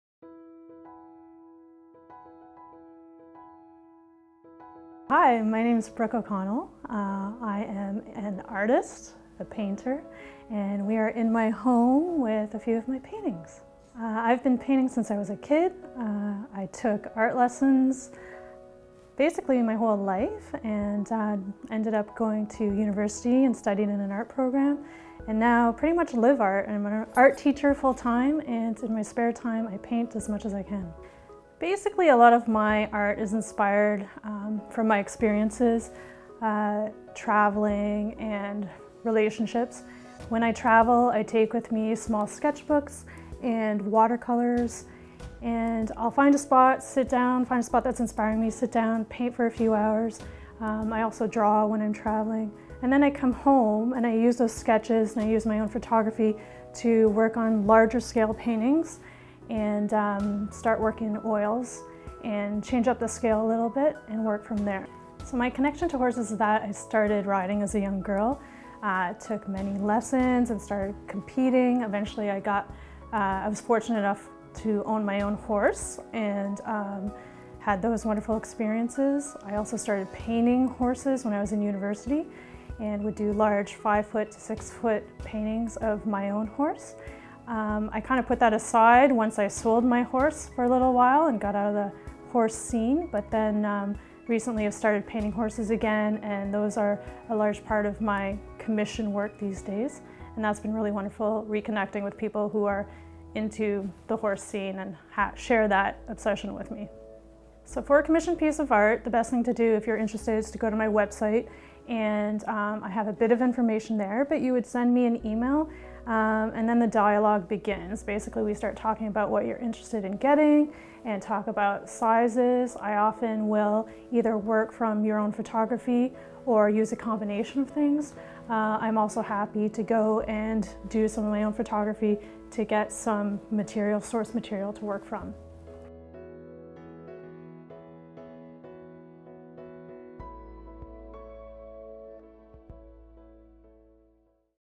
Ep 7: Brooke O'Connell
Brooke was born in Montreal, and has spent time living in both Canada and the United States. Although she has lived in many different places the artist considers St-Sauveur, a town in the Laurentian Mountains, her home.  Her love for drawing and painting began at an early age, inspired by time spent in the Laurentians working alongside her artist grandmother.

In 2001, Brooke graduated from McMaster University earning an Honors degree in Visual Arts. During her university years the artist spent her summers traveling in Arizona and British Columbia.  Many of her paintings have been a result of the inspiration found in the light and colours of such landscapes.

Brooke currently works as a visual arts teacher in the public school system.  Since becoming a teacher, her summers have been spent traveling and painting.  While traveling, her medium of choice is watercolour because they are lightweight, quick- drying, and immediate.  These watercolours often serve as preliminary studies for larger oil paintings.

The artist has exhibited in Ontario and Quebec, and has a number of works in private collections.  She currently resides in Burlington, Ontario, and continues to create and exhibit work based on travel, relationships, and personal experiences.

Burlington 52 always welcomes thoughts or ideas for stories, we can be reached through email in our contact section on our website.